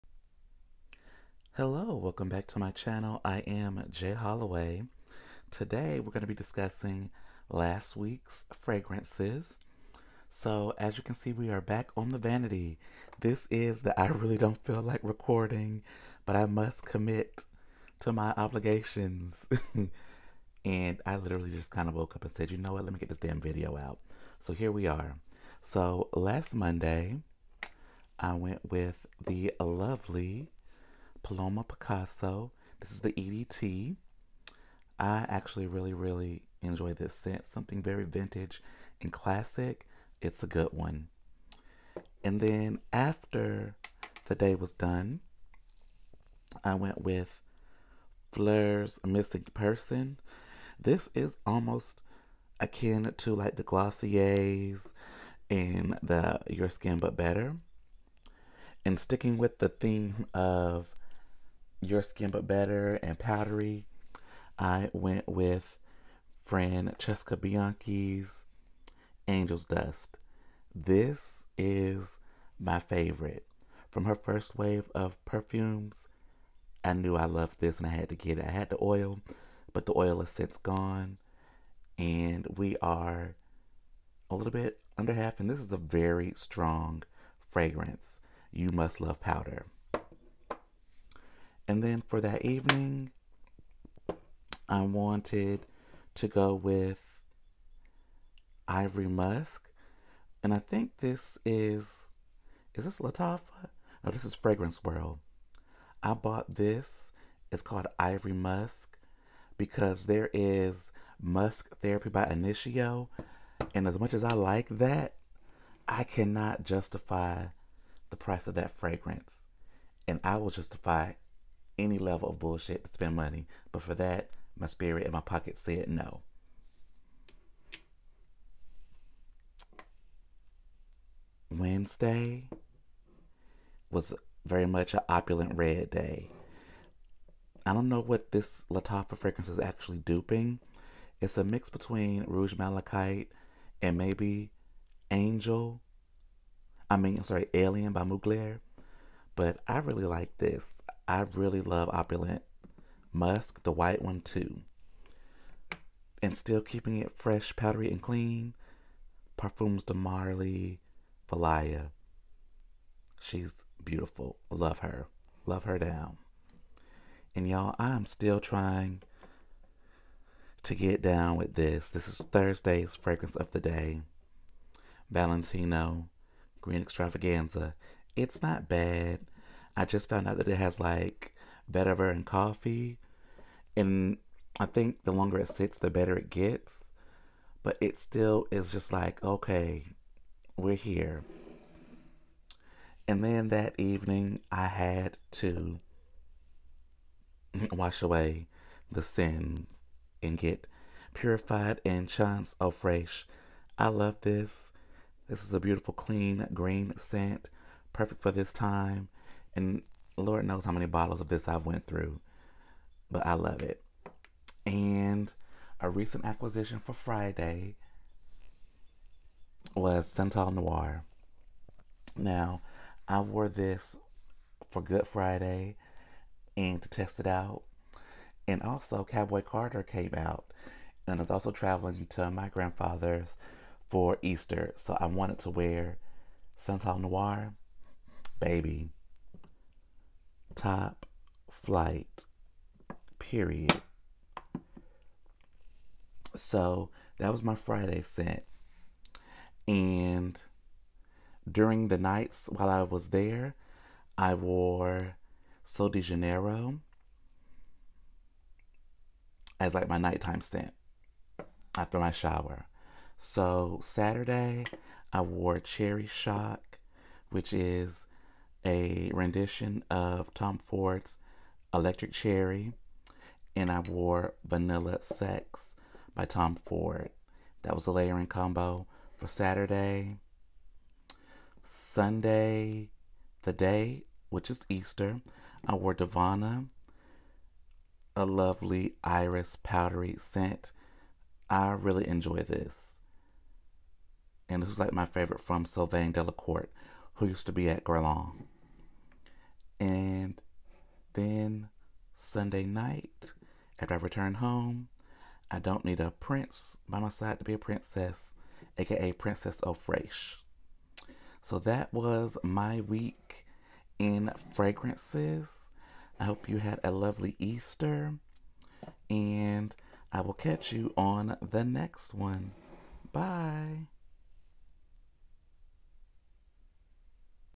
Weekly Fragrance Roundup 32
Last week was filled with joy! New music and family AND scents!!

Paloma Picasso
Missing Person
Angel’s Dust
Ivory Musk
Valaya
Valentino Green Stravaganza
Chance Eau Fraiche
Santal Noir
Vanilla Sex/Cherry shock
Dovanna
Cheriosa 59
Princess Eau Fraiche